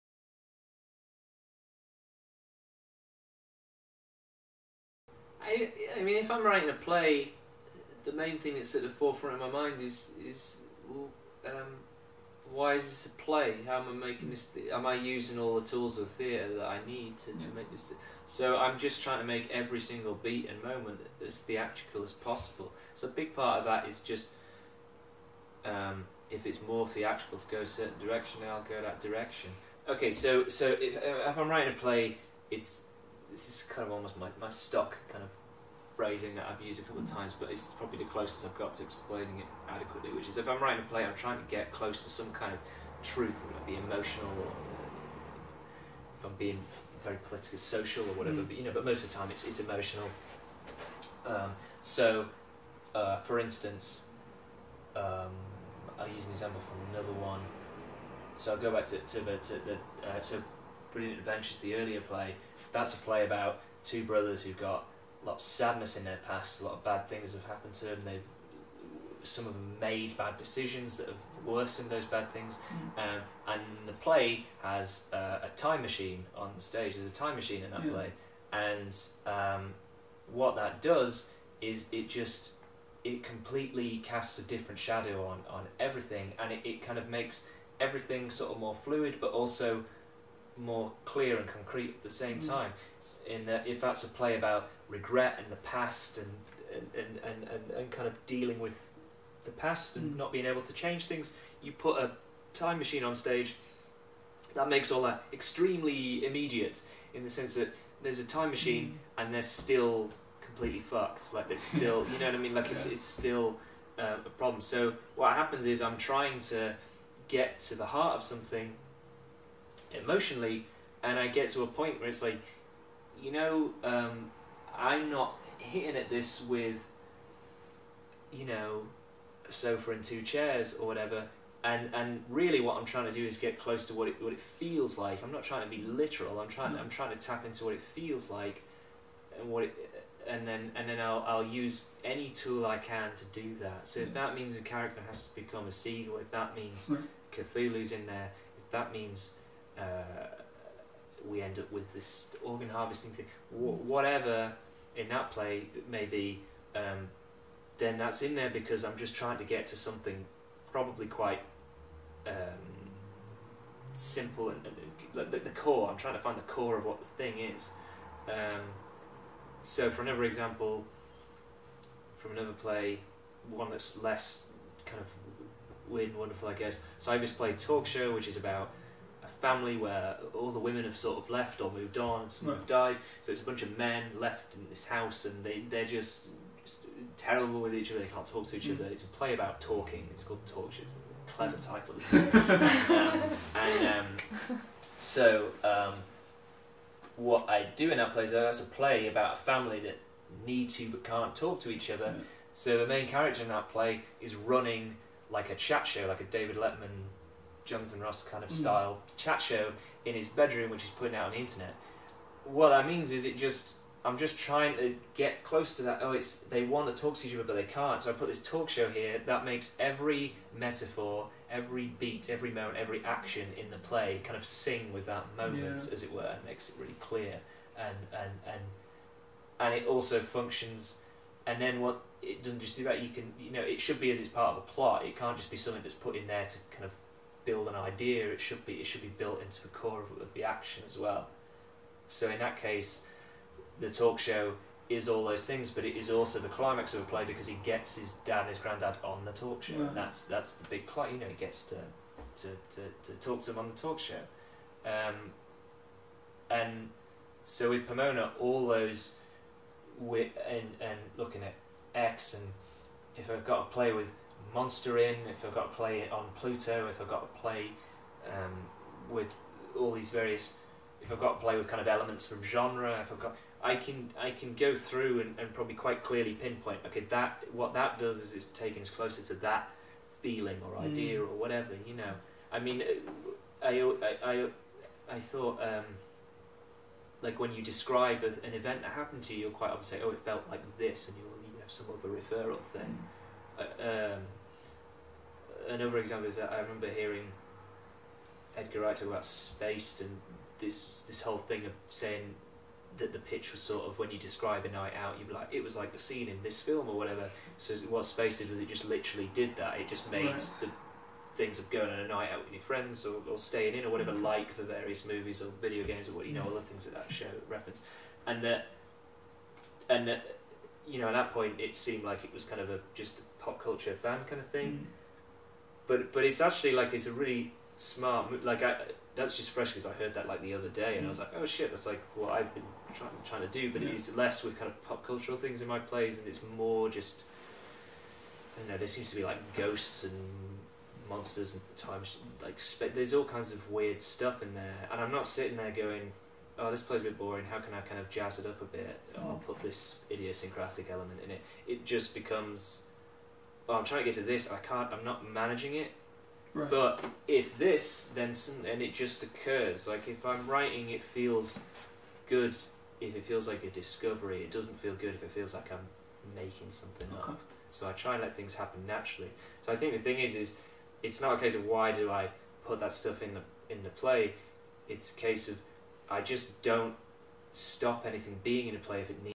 Exclusive Alistair McDowall - Part 7
Exclusive interview with playwright Alistair McDowall conducted by members of the Pomona company, being performed at University of York.

Part 7 - Desired Effects.